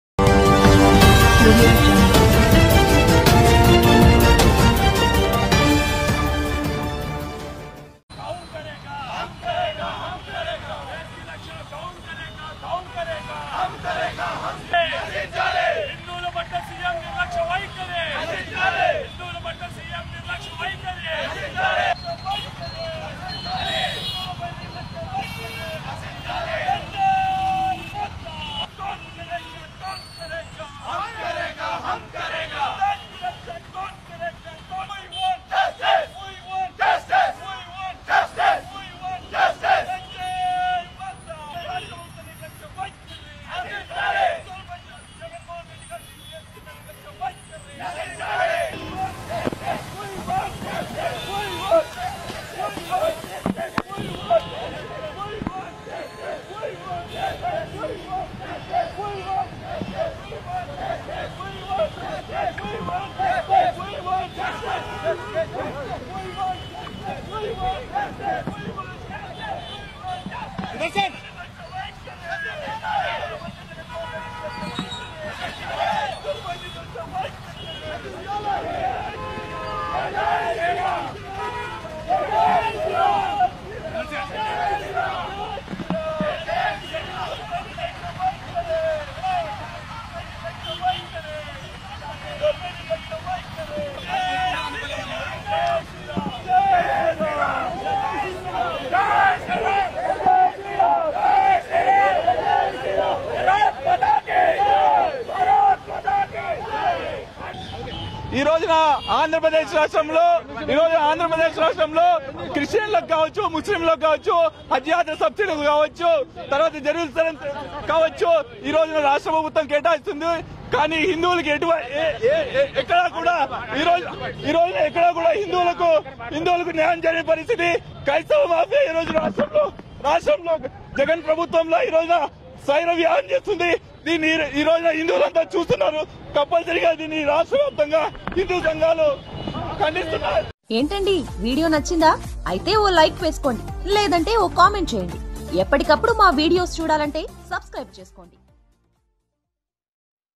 హిందువుల మనోభావాలను కించపరిచేలా వ్యవహరిస్తున్నా జగన్ పై రెచ్చిపోతున్న హిందువులు | Fata Fut News
Watch#